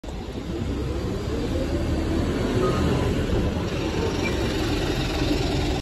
いすゞエルガミオ京成タウンバスTO55小54保育園・小岩駅北口経由京成小岩駅行き京成高砂駅12時54分発車5分遅れ
その乗務員は古本さんで11975です。
シルバーパスちやんと見せないと言われます。